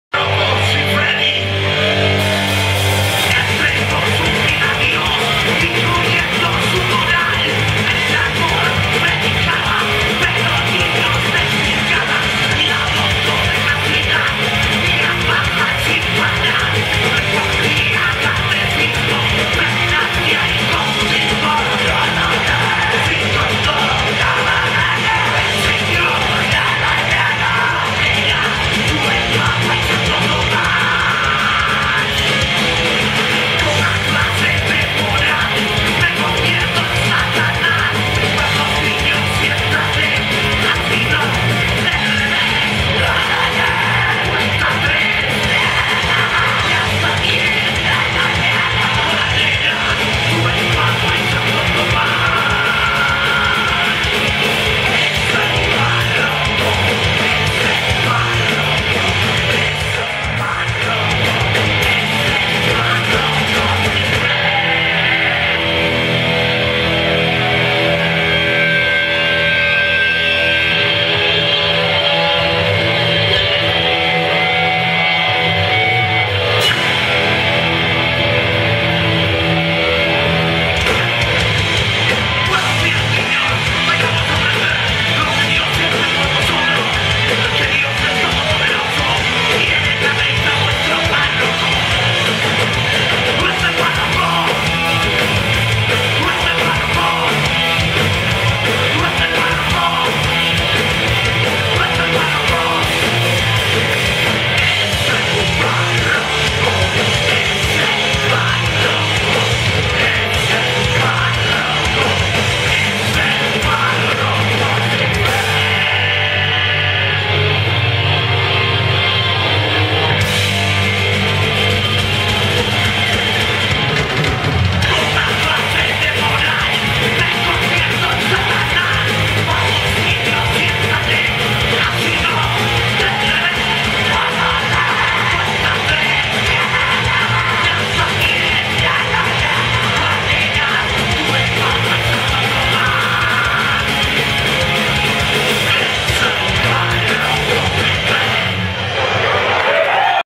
The Black Panthys Party - Directo Underpark Fest 2018
TBPP, Punkrock - Asturies

Underpark Fest 2018
Fecha: 25 / Agosto / 2018
Entrada: Gratuita
Lugar: Lugones (Asturias)
Bandas: + The Plastic Hammers + Flashback + Bates Motel + Fre City + Blowfuse